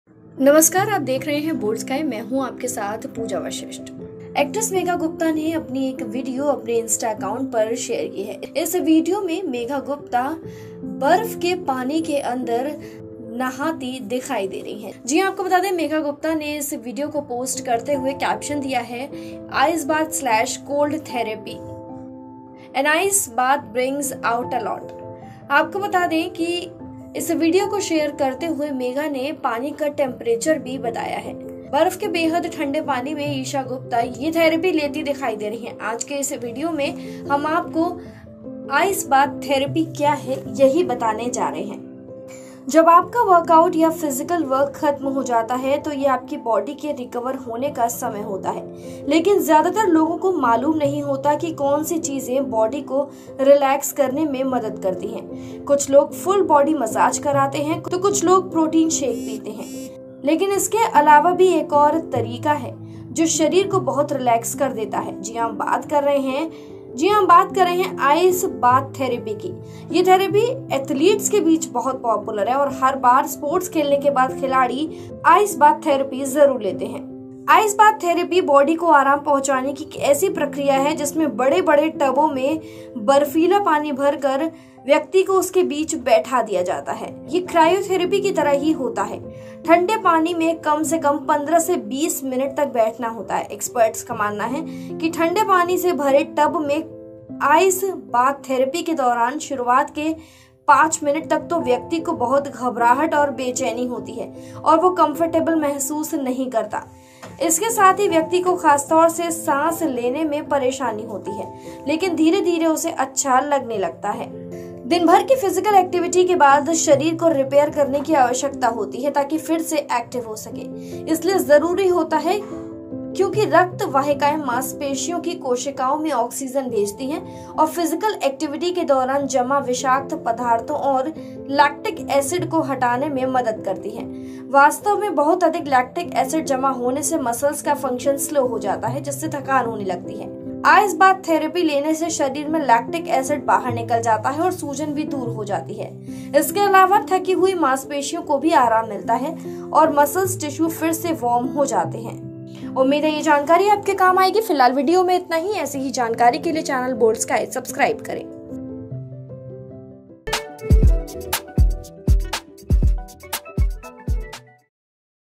Ice Bath Therepy क्या है? Ice Bath Therepy Benefits । Boldsky *Health
Actress Megha Gupta has shared a video on her Instagram account taking ice bath therapy, in such a situation people want to know what is this ice bath therapy too What are the benefits of Ice Bath therapy

एक्ट्रेस मेघा गुप्ता ने अपने इंस्टाग्राम अकाउंट पर आइस बाथ थेरेपी लेते हुए एक वीडियो शेयर किया है ऐसे में लोग जाना चाहते हैं कि आखिर यह आइस बाथ थेरेपी भी क्या होती है आज कैसे वीडियो में हम आपको आइस बाथ थेरेपी क्या है और इस आइस बाथ थेरेपी के क्या फायदे हैं यही बताने जा रहे हैं